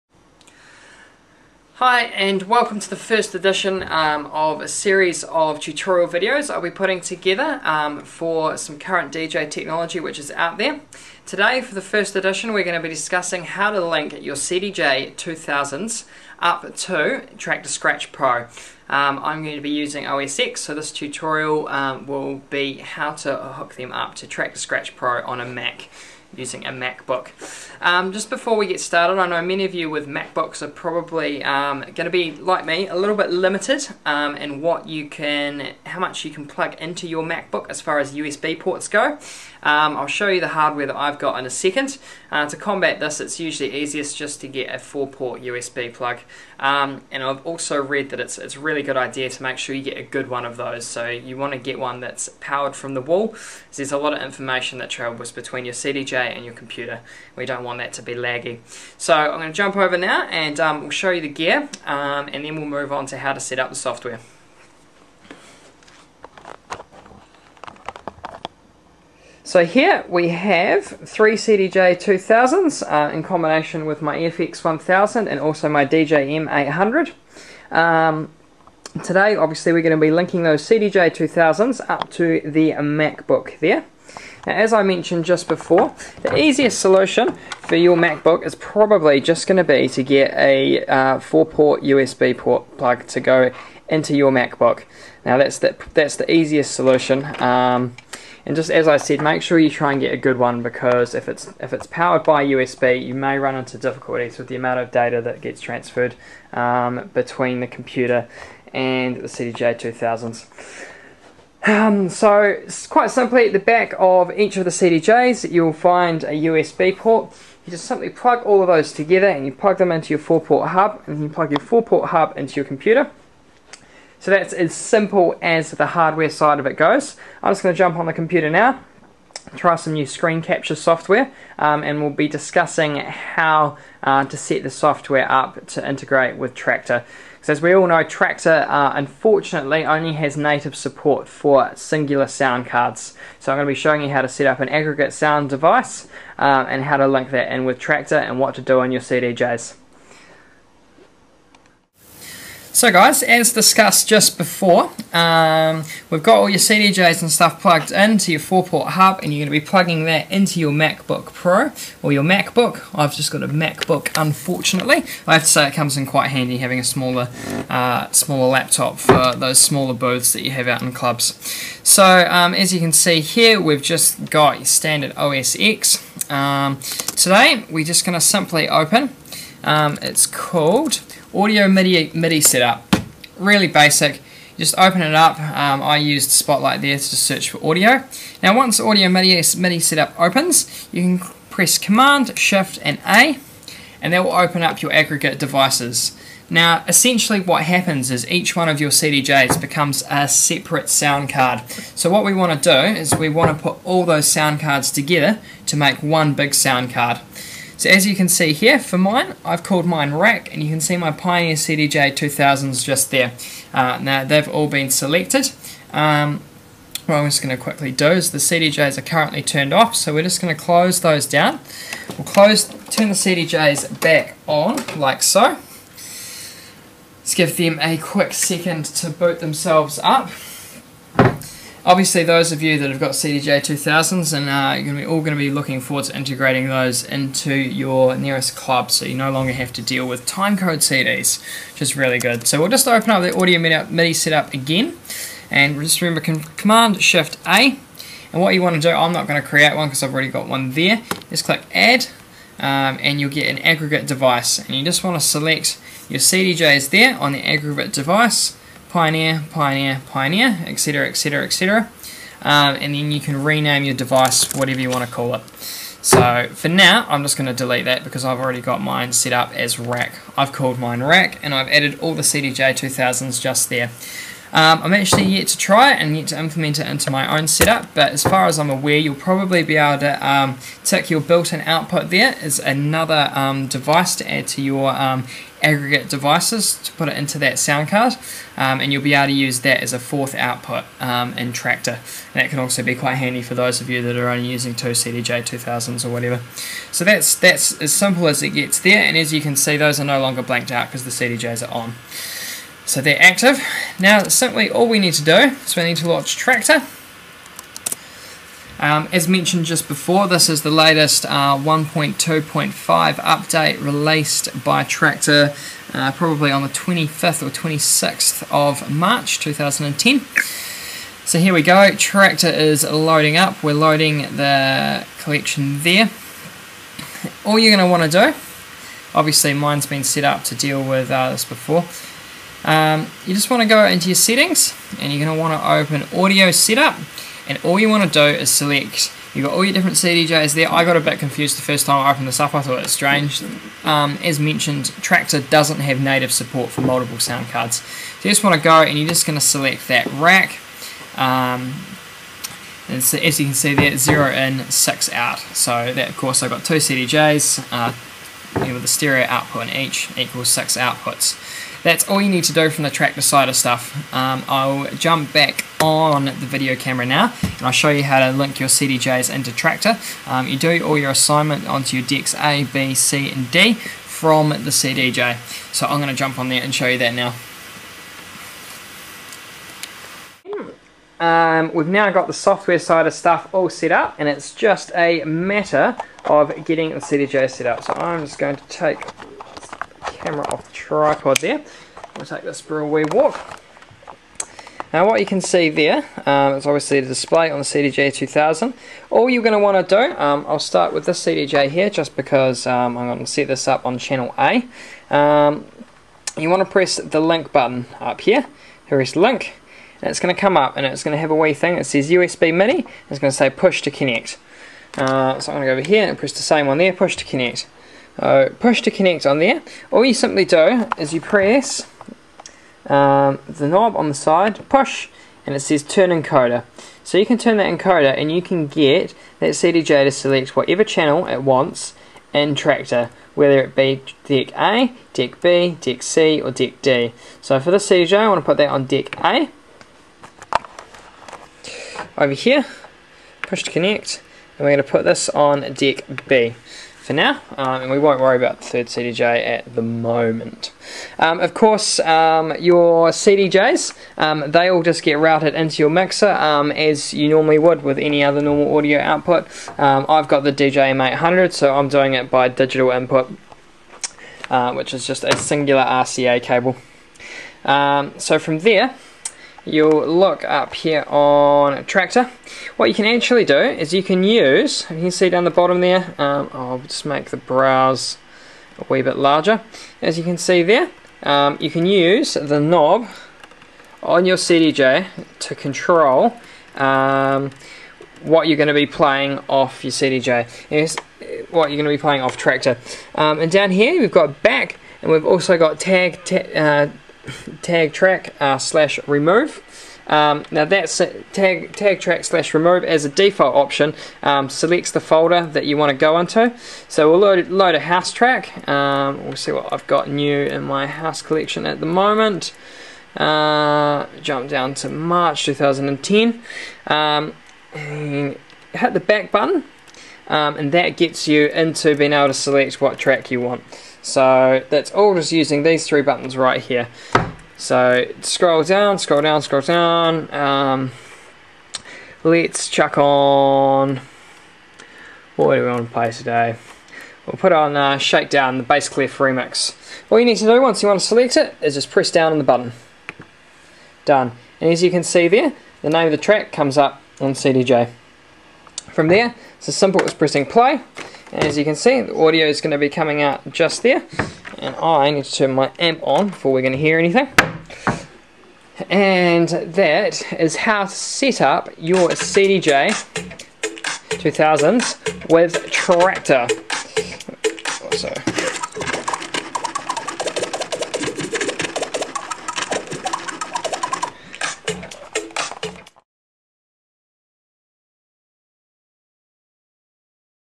TUTORIAL - Setup CDJ2000s with Traktor Scratch Pro
Tutorial with Sebastian Wild. A slightly more comprehensive video on how to setup your CDJ2000's and Traktor Scratch Pro with OS X using aggregate sound devices. Also includes a quick introduction to some of the track selection features enabled by default between the two.